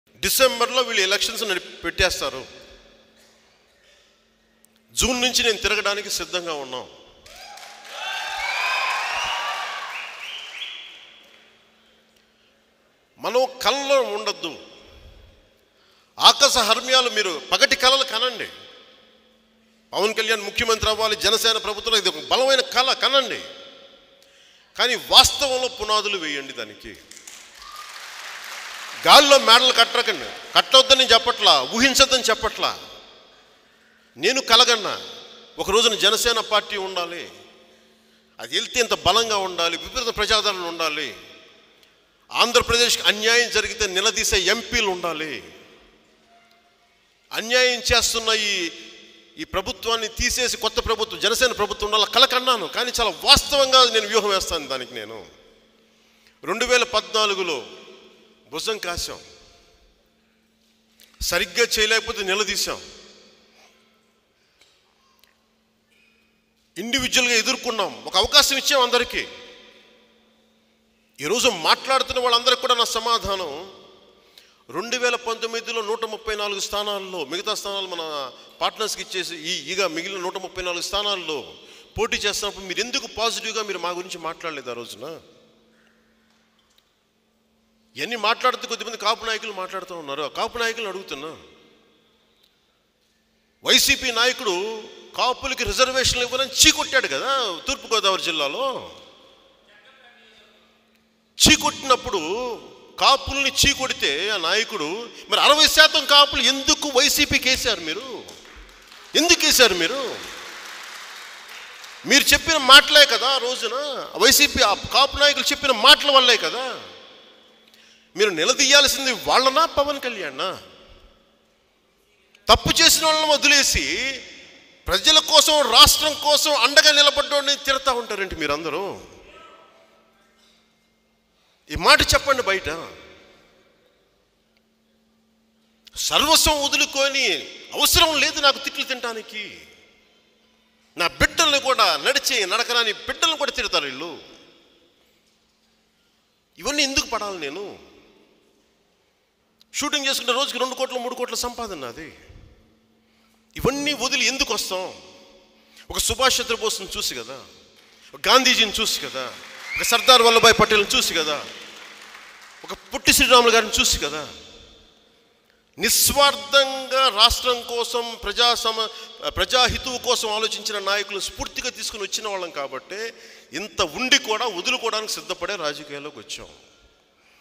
ఒక్కరోజుకి 2 నుండి 3 కోట్లు ఇస్తారు | Pawan Kalyan Reveals His Remunaration Per Day | ZIP TV
ఒక్కరోజుకి 2 నుండి 3 కోట్లు ఇస్తారు | Pawan Kalyan Reveals His Remunaration Per Day | Pawan Kalyan Speech At Mangalagiri Meeting | @ziptvprime

6) ZIP TV Money